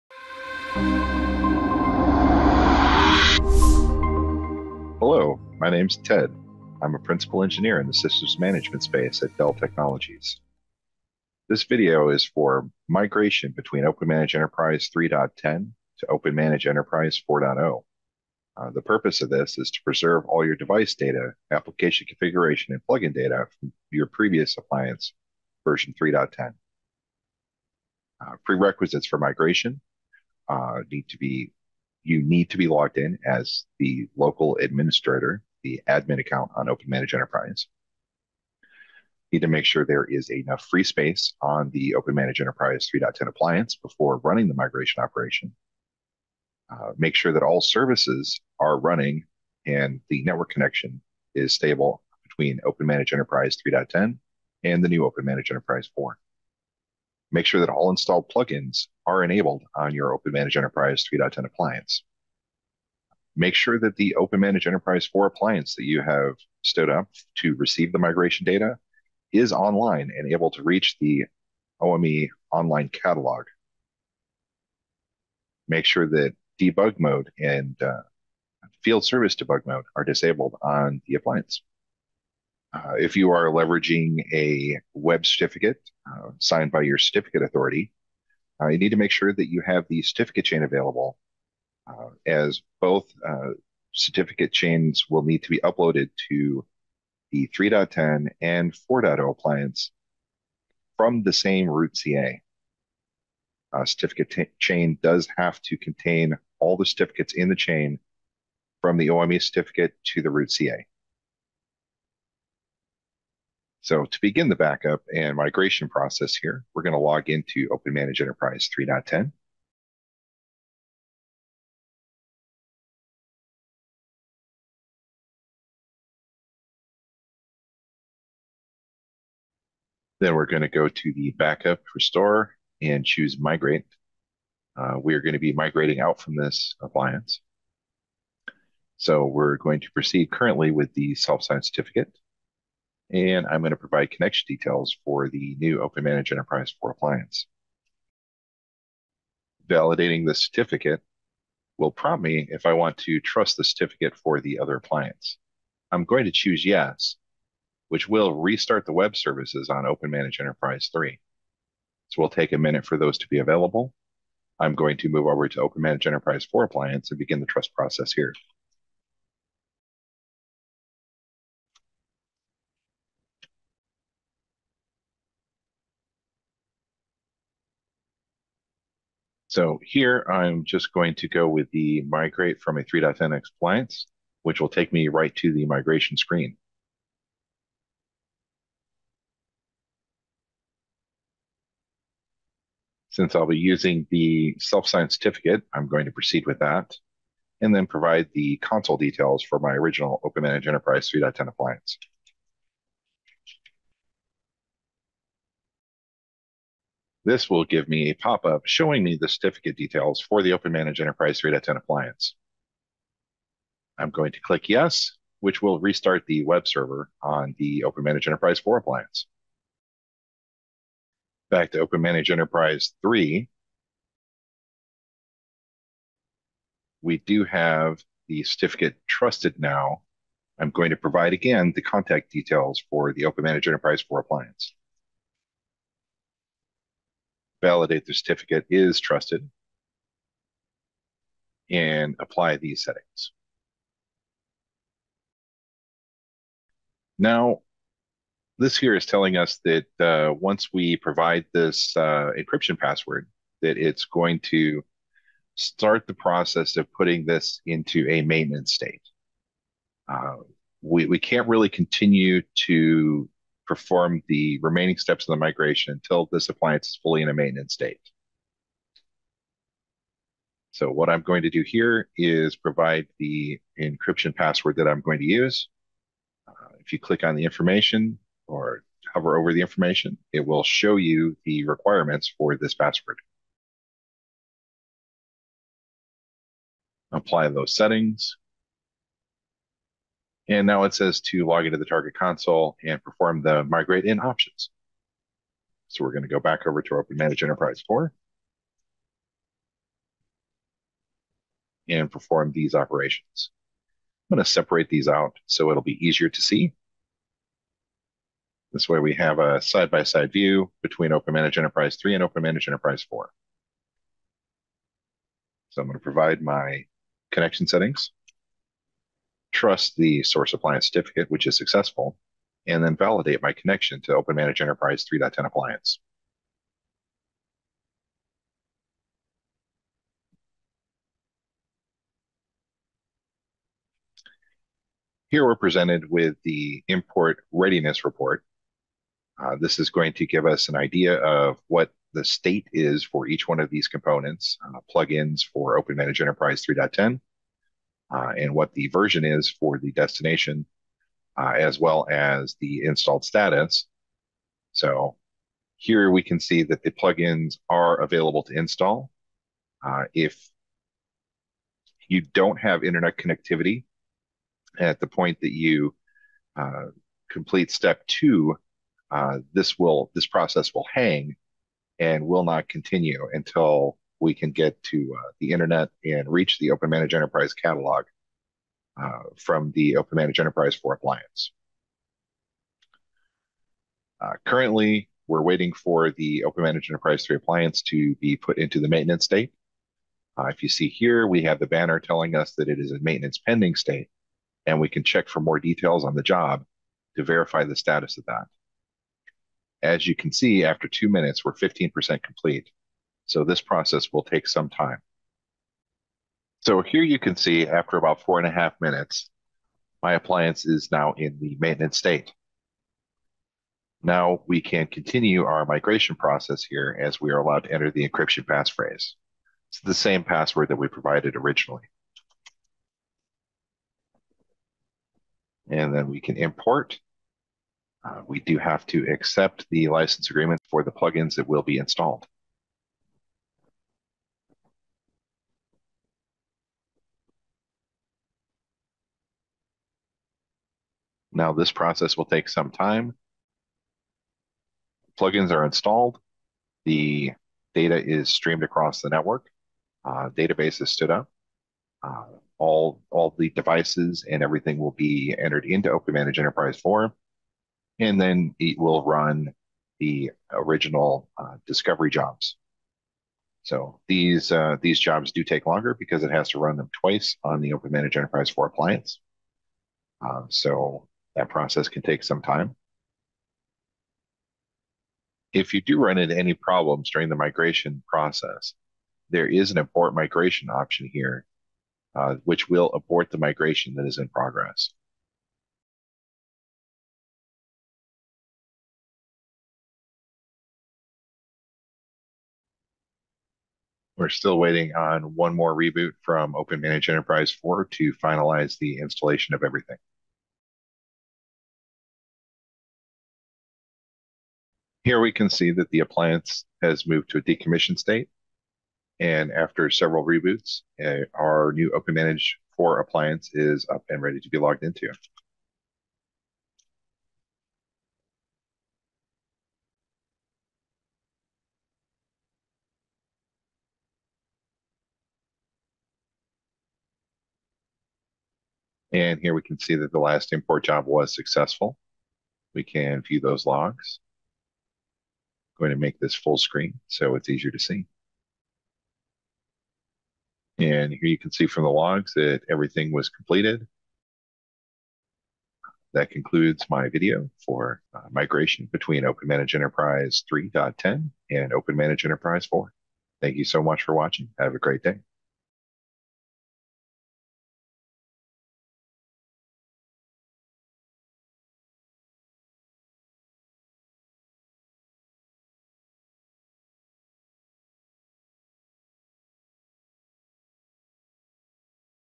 How to Migrate OpenManage Enterprise From 3.10.x to 4.0
Here's how to migrate OpenManage Enterprise from 3.10.x to 4.0